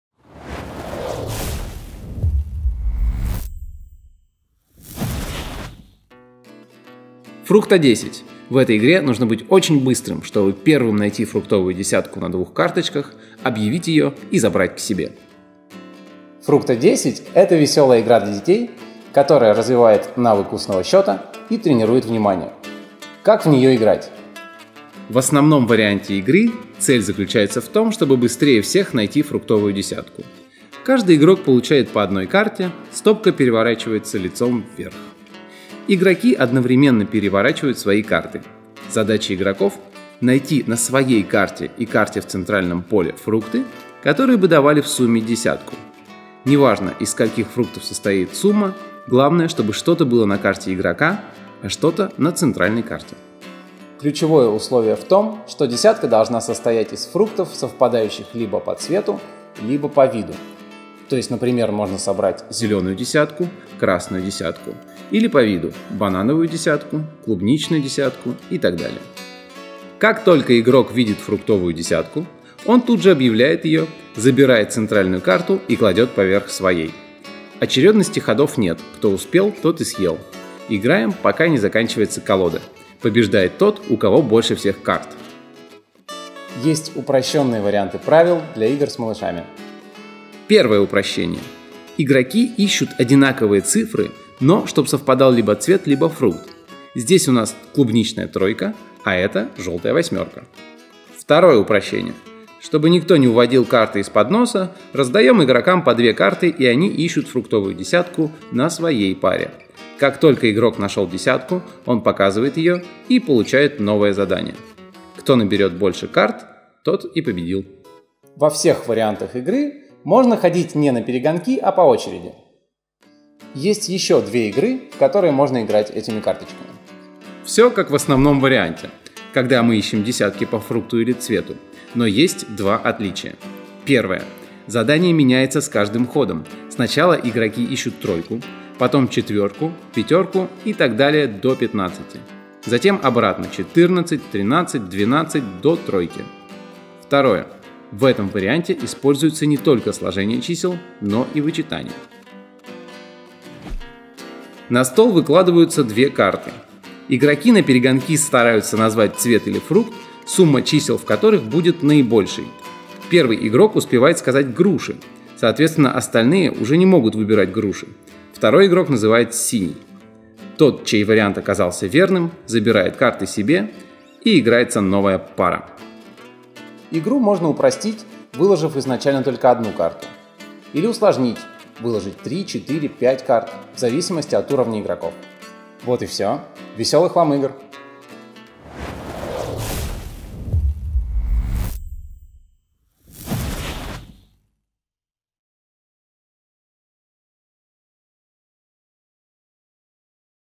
NEW! Фрукто 10 видеоинструкция к игре от Банды Умников
Эта игра на развитие навыка скоростного сложения, внимательность и умение концентрироваться не оставит равнодушных среди детей и родителей. В правилах вы найдёте уйму разнообразных вариаций игры – от самого простого до очень сложного